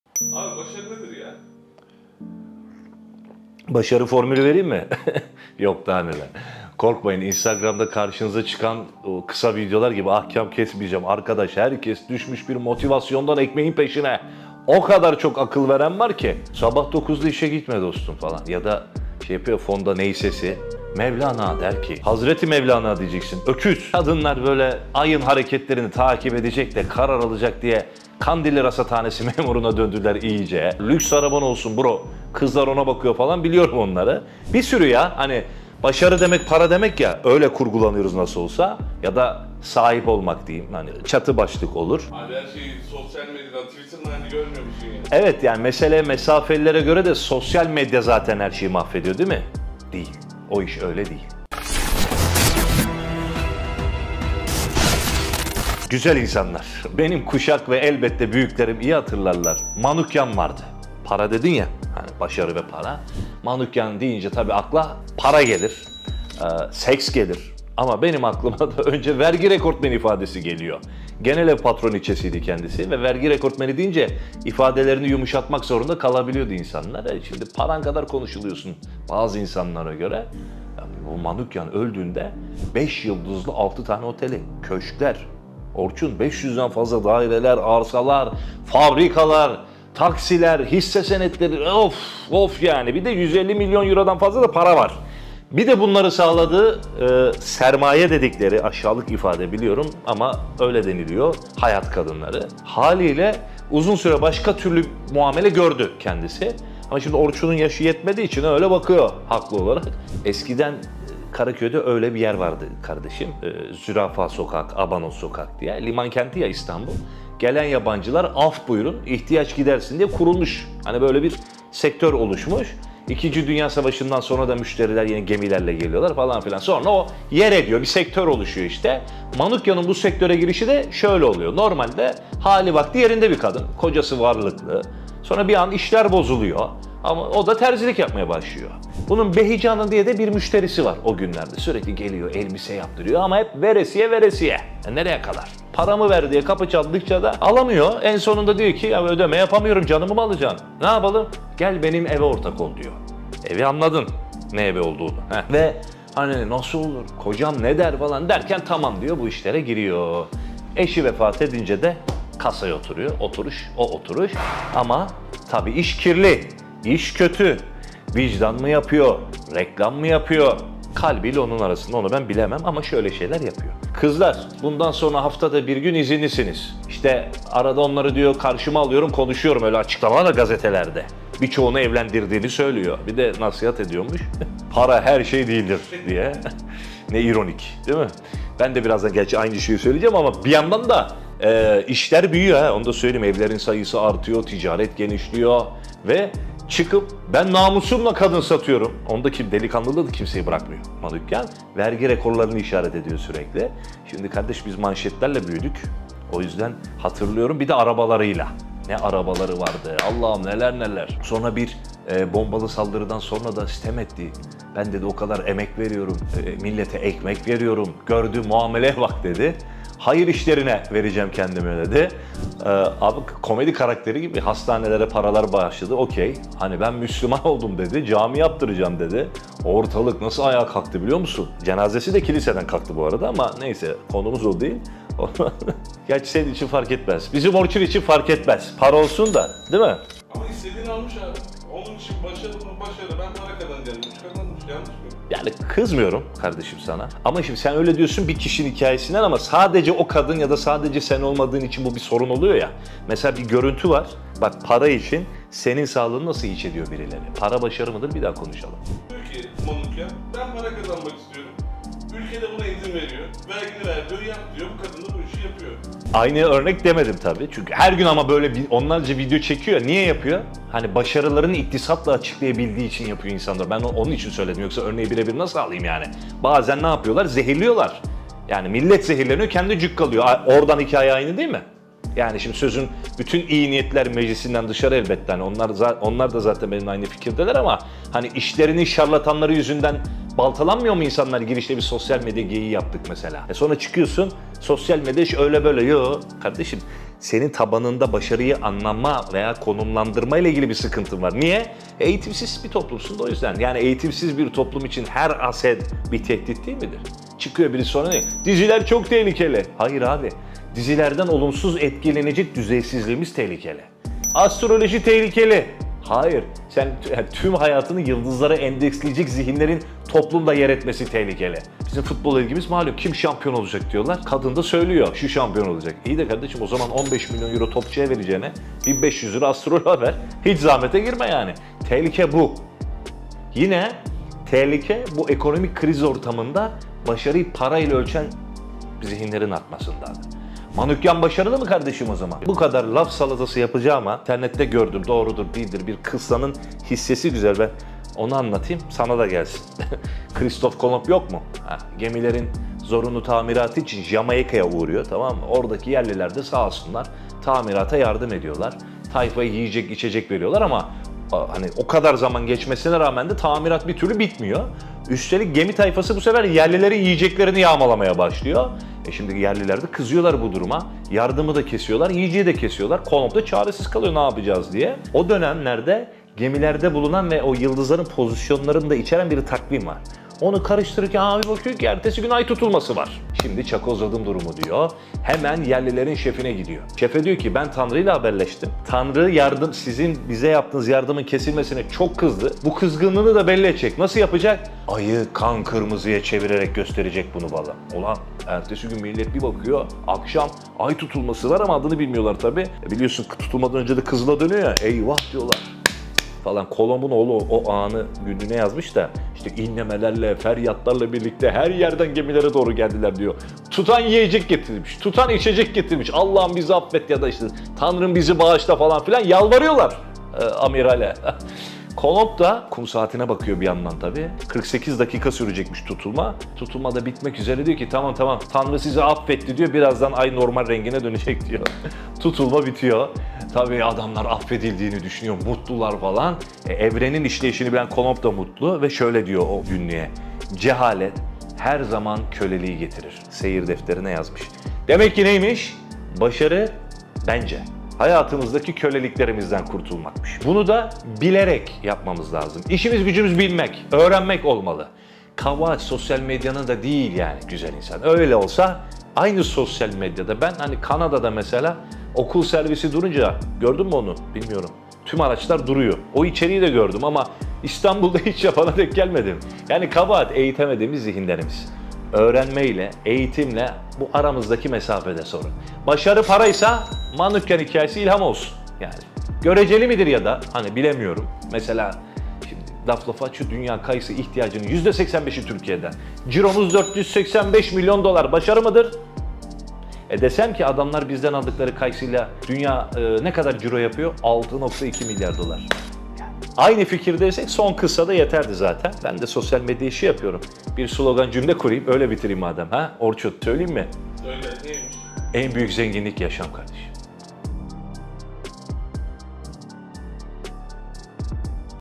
Genelev Patroniçesi Matild Manukyan Nasıl Başardı?
Güzel insanlar, öyle hayatlar var ki öğrendikçe insanın aklı şaşıyor.

Genel ev partoniçesi Matild Manukyan'ın ilginç hayatı da onlardan birisi. Peki bir dönemin vergi rekortmeni Manukyan kimdir?  Neden ve nasıl genel ev patroniçesi oldu? Yaptıkları başarı mı yoksa utanç mı?

Ufuk Kaan Karacan'ın anlatımıyla 'O İş Öyle Değil!' sizlerle.

İyi seyirler.